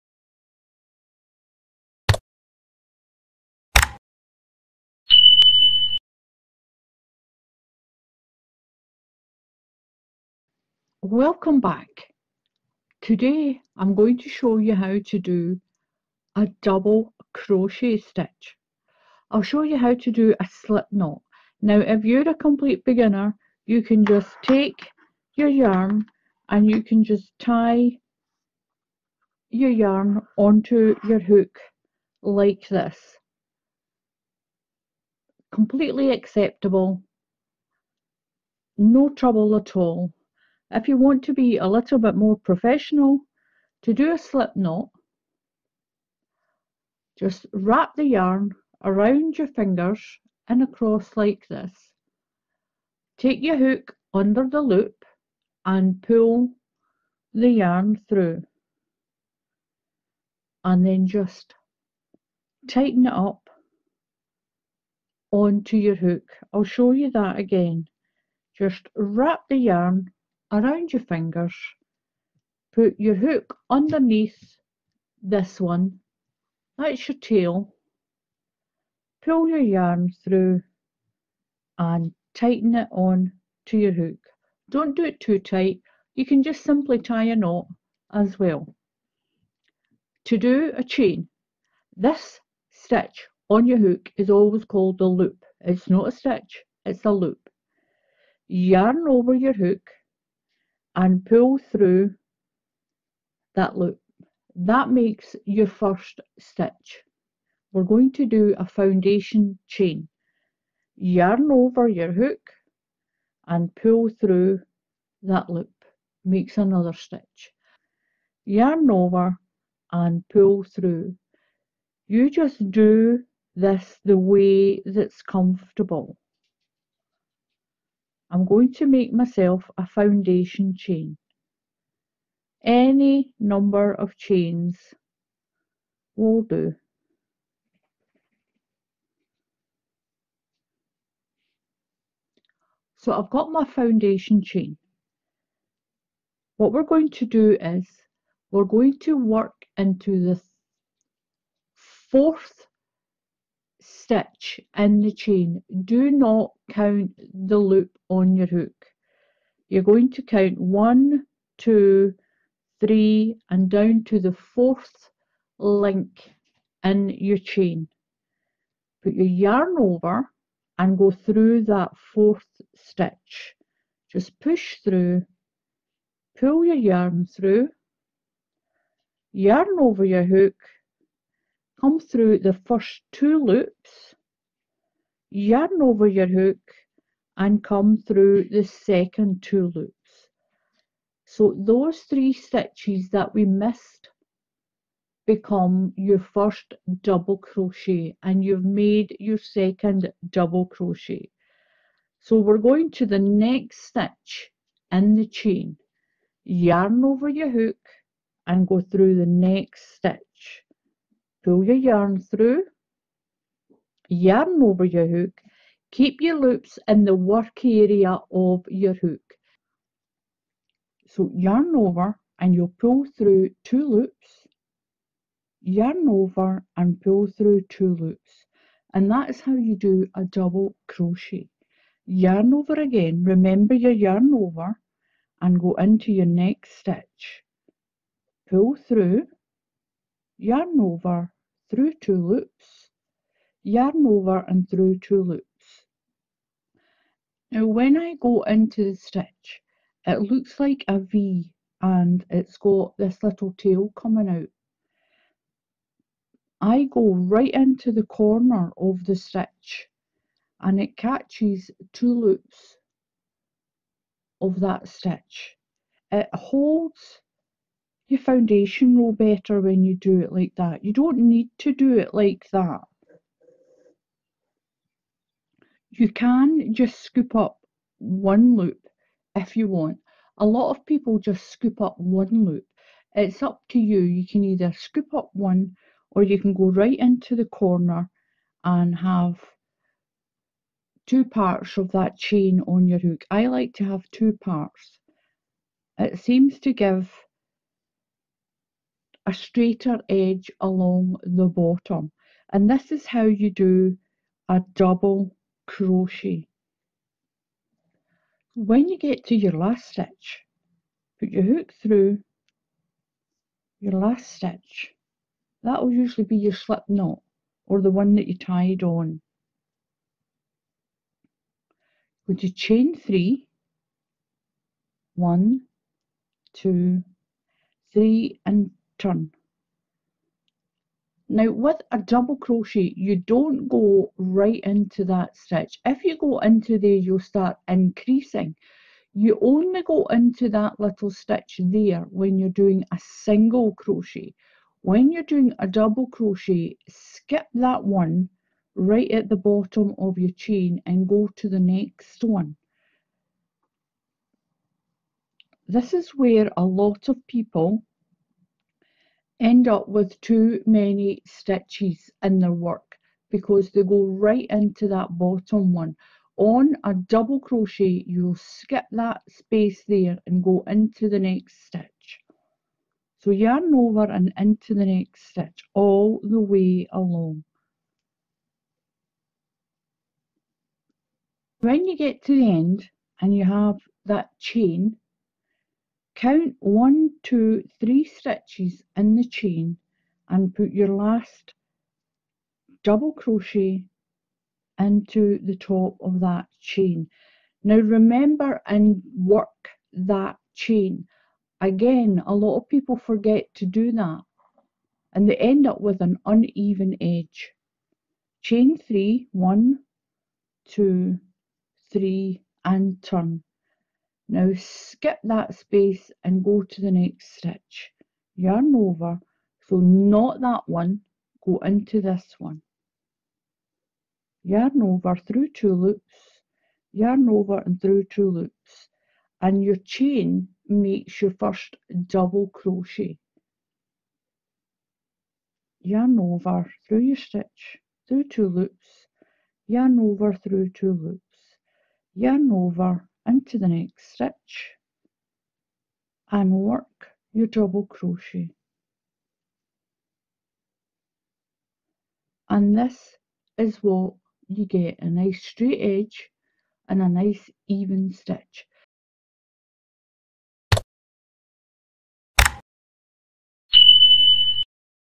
Left handed crochet for Beginners. Easy Double crochet stitch.
LEFT HANDED crochet for beginners. USA double crochet stitch.  No idle chat Just the crochet. This quick easy crochet tutorial will have anyone new to crochet making a project in no time. This simple no nonsense tutorial is quick and easy. Double crochet for left handed people.